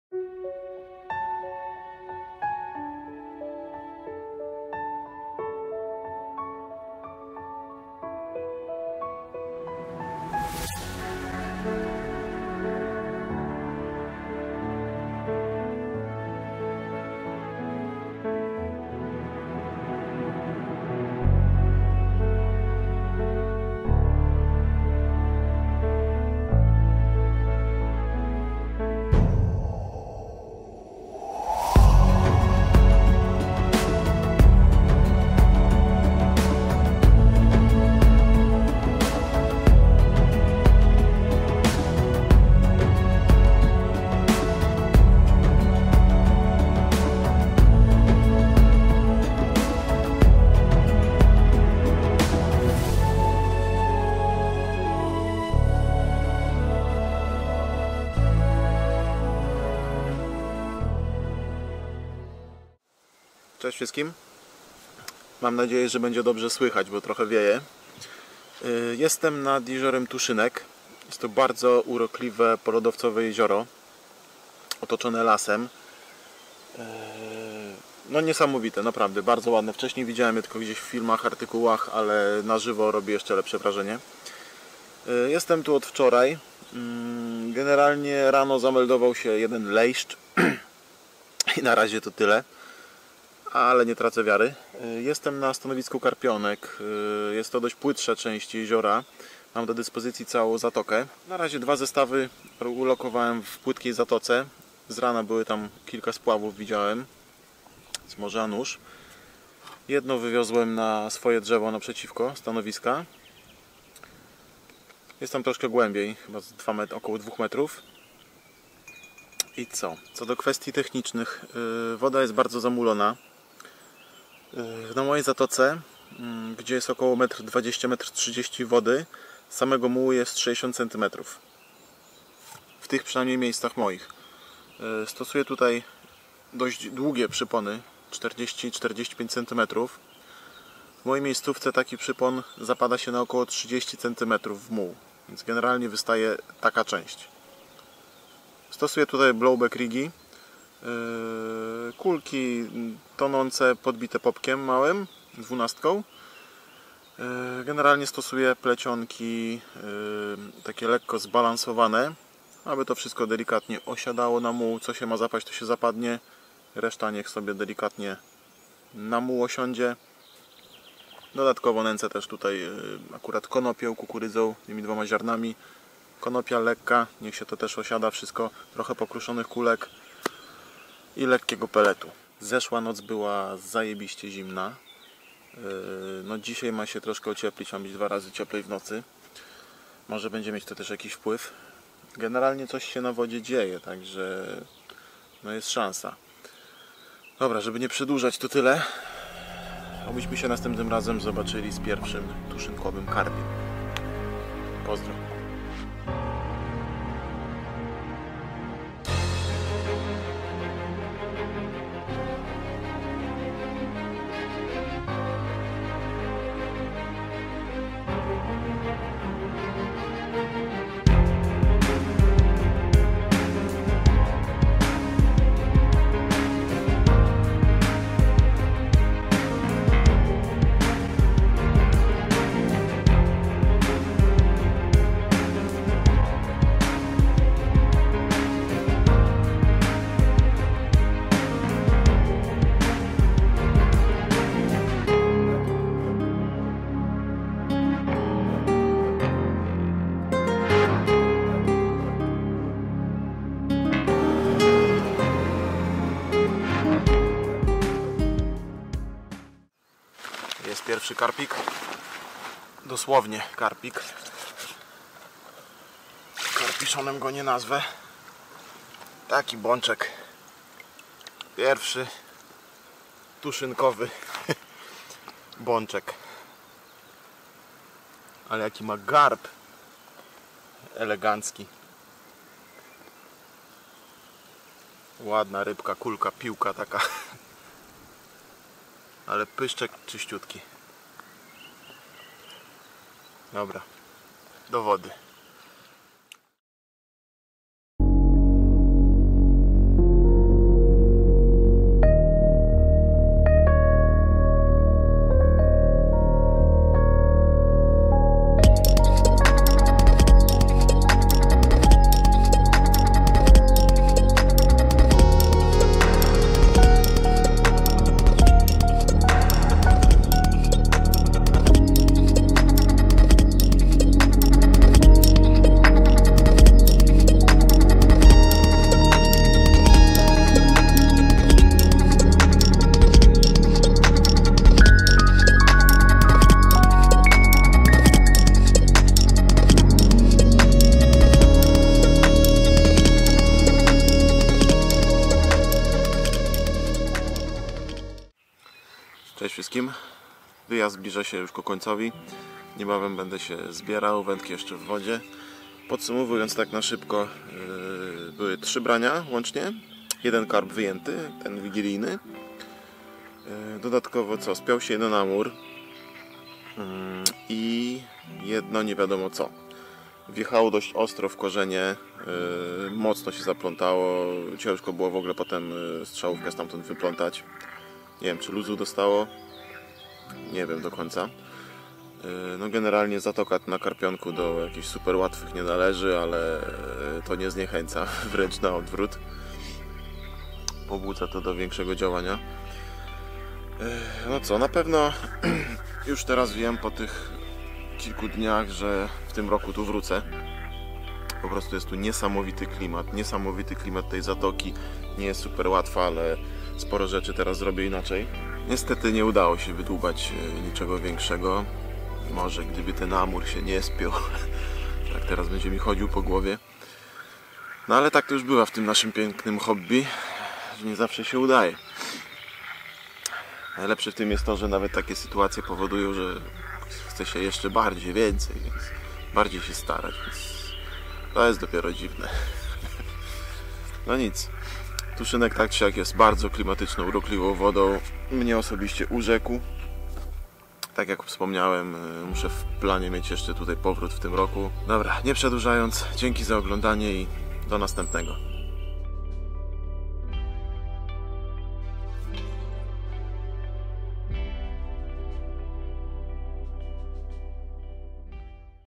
Zamulony Tuszynek cz.1
Kilkudniowy rekonesans nad jeziorem Tuszynek. Woda niesamowicie urokliwa. Niedosyt jaki pozostał poskutkuje zapewne kolejnymi odwiedzinami, taka to już ta nasza karpiowa choroba ;)
Pozdro i do zobaczenia nad wodą.